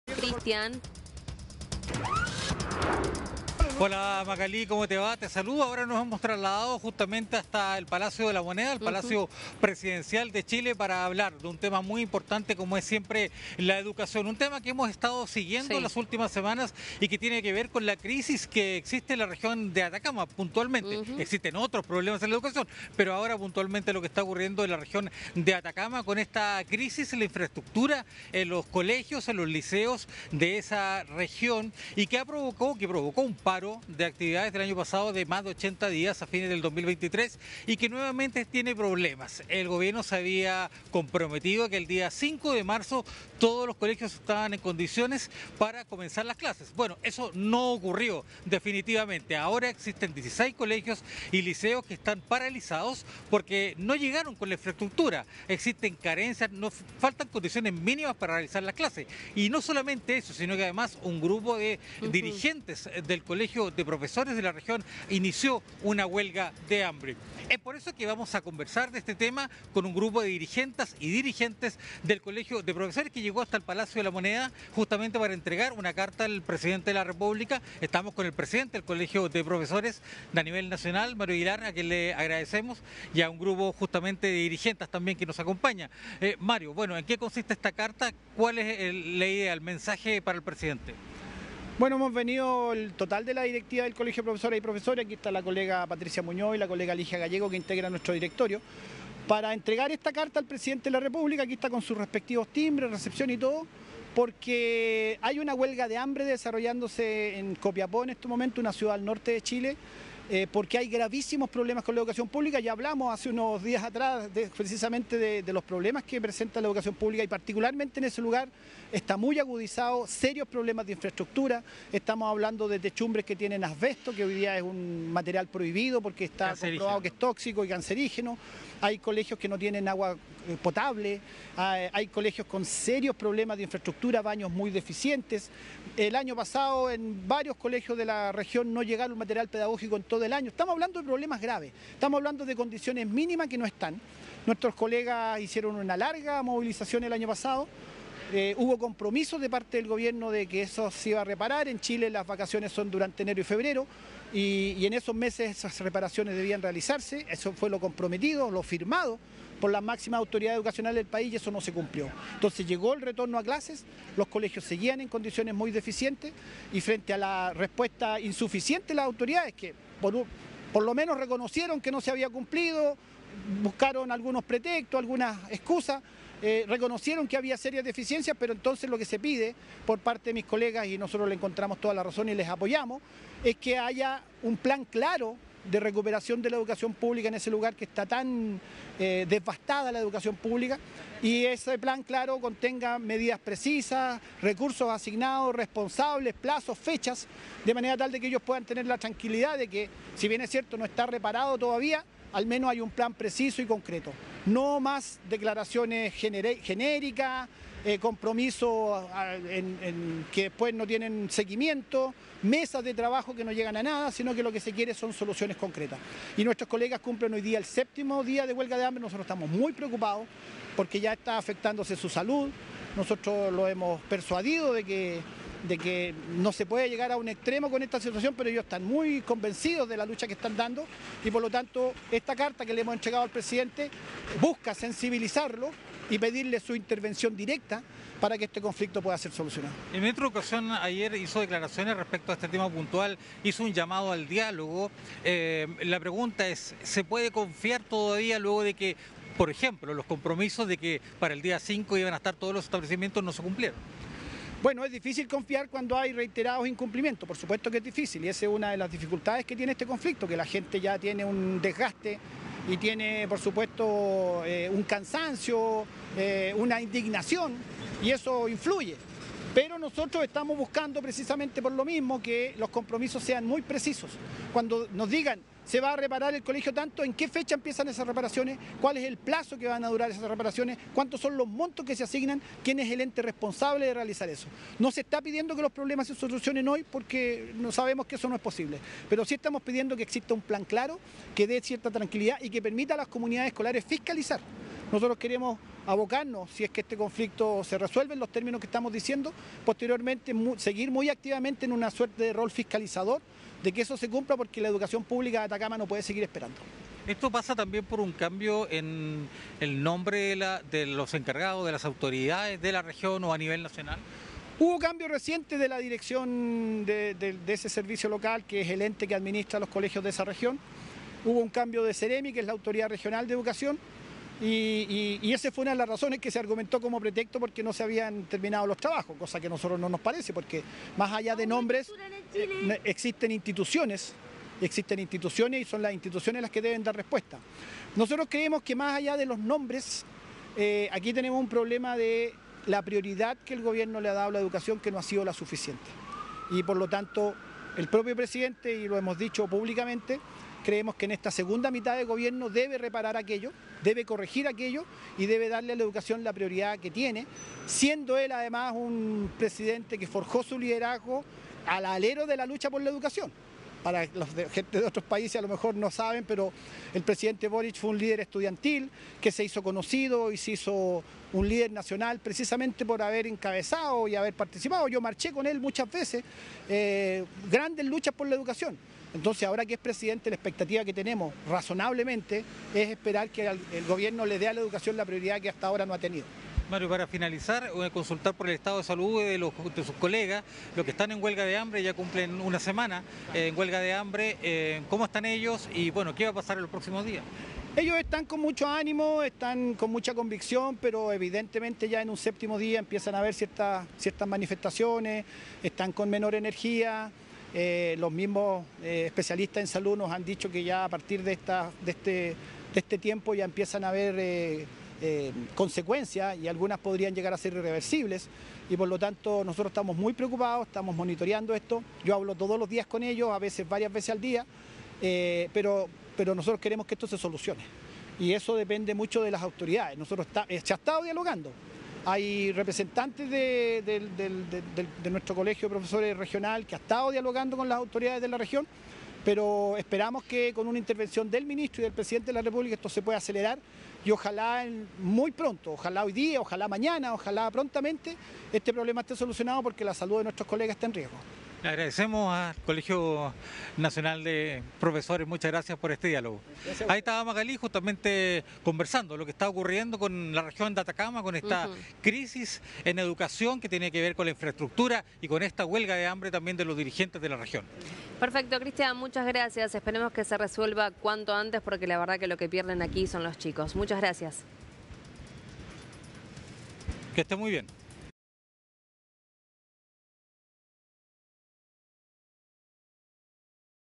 [DNEWS] CHILE: "HAY UNA HUELGA DE HAMBRE DESARROLLÁNDOSE" MARIO AGUILAR
El medio de comunicaciones DNEWS entrevistó al presidente del Gremio Docente, Mario Aguilar, respecto de la carta entregada por el Magisterio esta mañana al mandatario Gabriel Boric por la grave crisis que viven las comunidades educativas de Atacama y que mantiene a Dirigente el huelga de hambre.